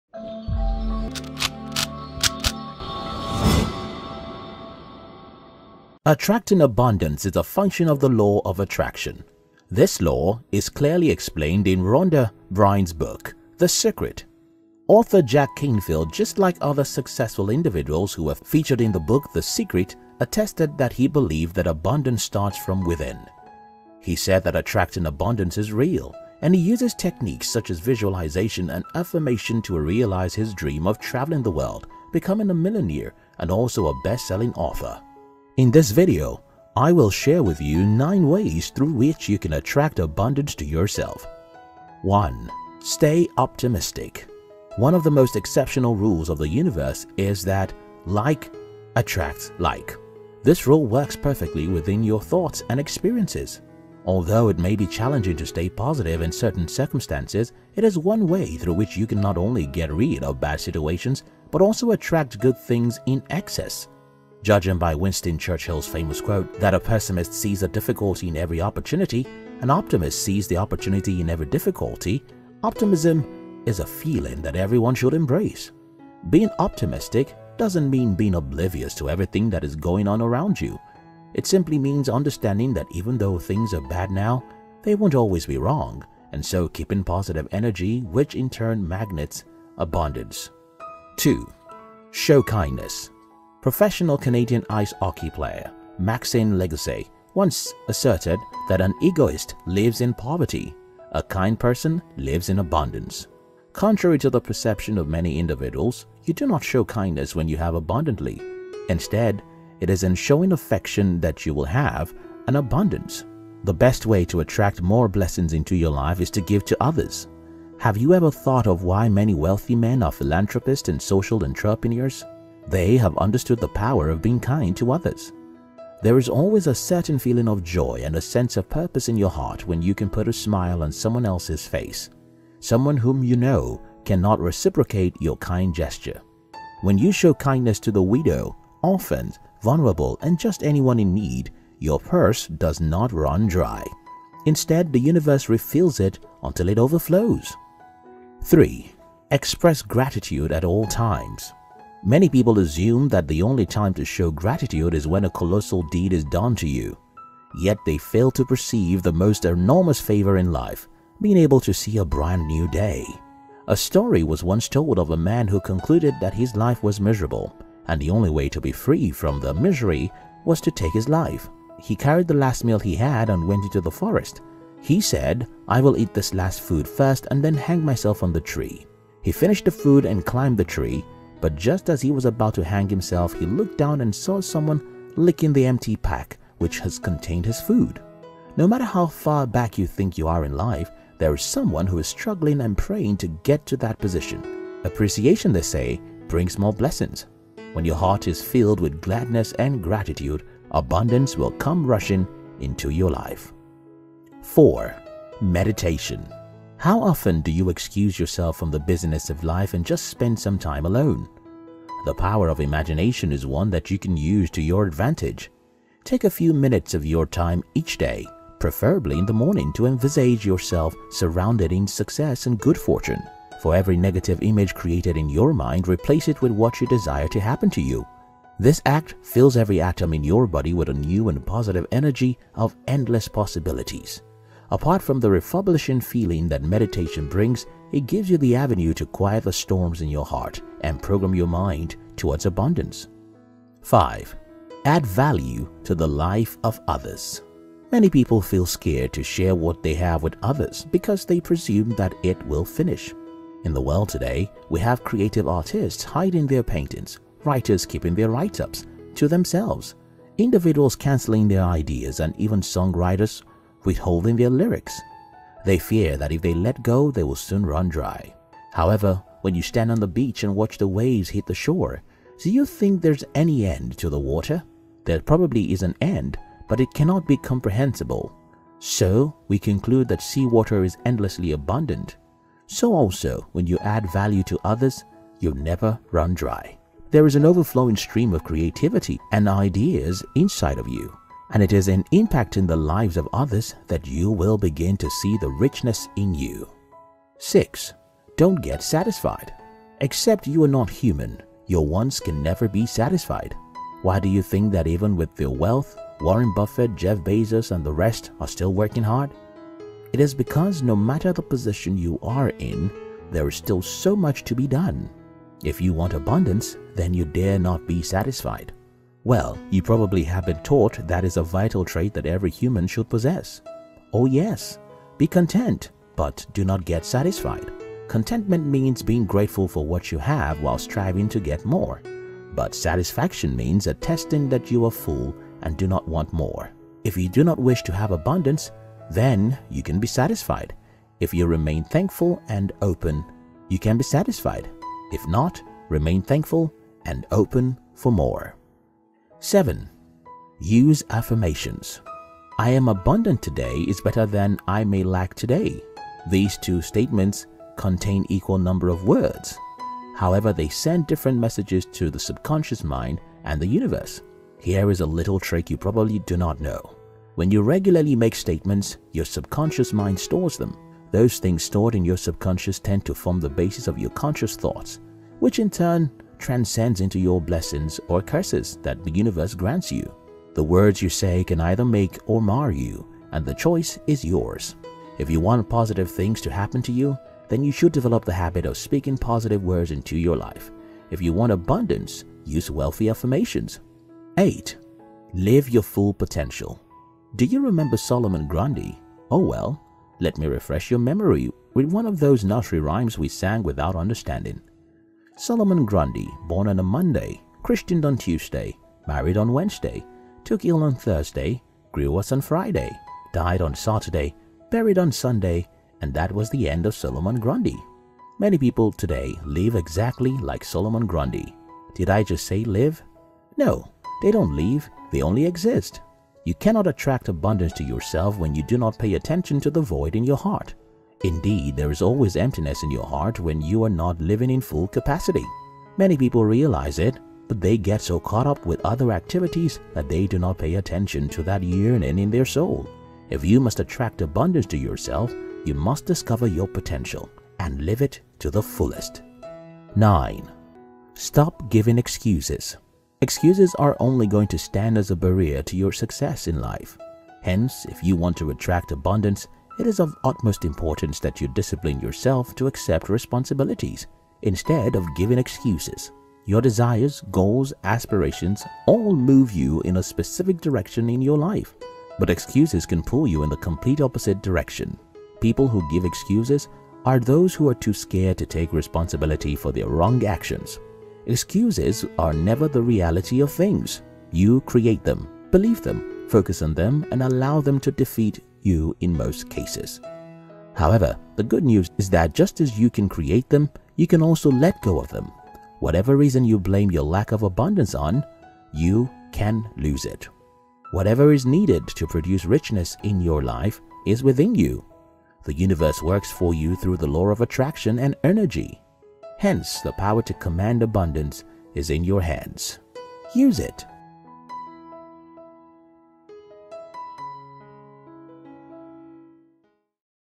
9 Ways to Attract Abundance in Your Life
Attracting abundance is a function of the law of attraction. This law is clearly explained in Rhonda Byrne's book, the secret. Author Jack Canfield just like other successful individuals who were featured in the book the secret attested that he believed that abundance starts from within. He said that attracting abundance is real, and he uses techniques such as visualization and affirmation to realize his dream of travelling the world, becoming a millionaire, and also a best-selling author.